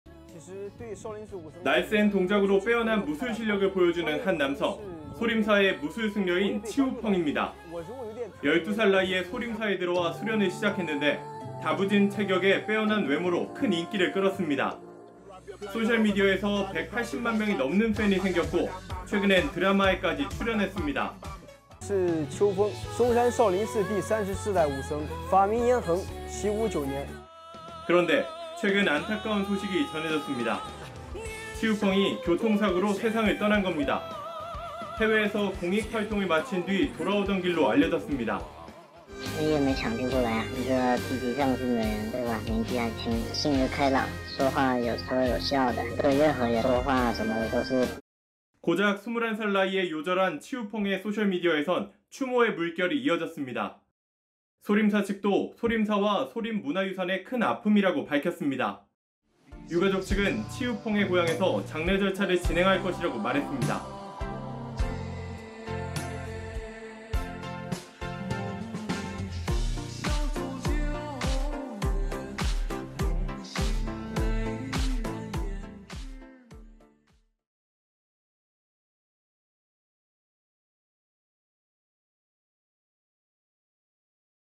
"소림사 꽃미남 스님" 인기몰이했는데…21살에 갑작스런 사망 소식 '충격'｜지금 이 뉴스
중국에서 ‘가장 잘생긴 승려’로 인기를 끌어온 소림사 무술 승려 치우펑이 최근 교통사고로 숨졌다고 중국 매체들이 보도했습니다. 9년 전 소림사에 들어와 무술을 수련해 온 치우펑은 다부진 체력과 빼어난 미모로 유명해졌습니다. 소셜미디어에선 180만 명이 넘는 팬이 생겼고 드라마에도 출연했는데요. 해외에서 공익 활동을 마치고 복귀하는 길에 변을 당해 안타까움을 더했습니다. 영상으로 보시죠.